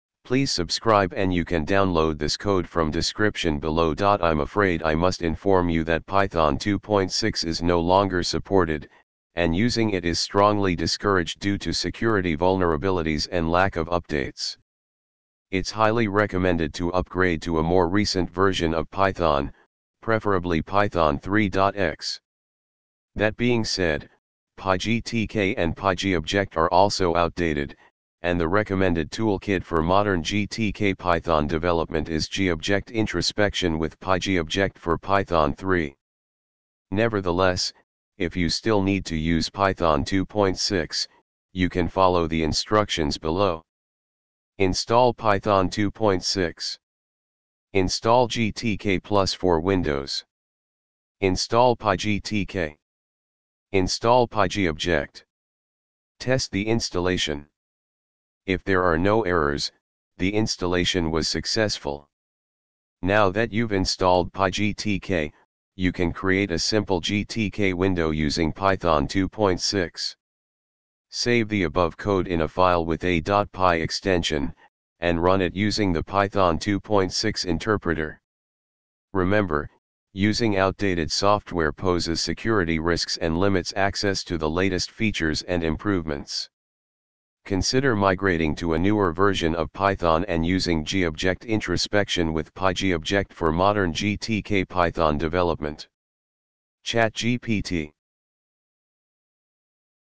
How do I install PyGTK PyGobject on Windows with Python 2 6
I'm afraid I must inform you that Python 2.6 is no longer supported, and using it is strongly discouraged due to security vulnerabilities and lack of updates. It's highly recommended to upgrade to a more recent version of Python, preferably Python 3.x.
That being said, PyGTK and PyGObject are also outdated, and the recommended toolkit for modern GTK Python development is GObject Introspection with PyGObject for Python 3. Nevertheless, if you still need to use Python 2.6, you can follow the instructions below.
Install Python 2.6:
Install GTK+ for Windows:
Install PyGTK:
Install PyGObject:
Test the Installation:
If there are no errors, the installation was successful.
Now that you've installed PyGTK, you can create a simple GTK window using Python 2.6:
Save the above code in a file with a .py extension, and run it using the Python 2.6 interpreter.
Remember, using outdated software poses security risks and limits access to the latest features and improvements. Consider migrating to a newer version of Python and using GObject Introspection with PyGObject for modern GTK Python development.
ChatGPT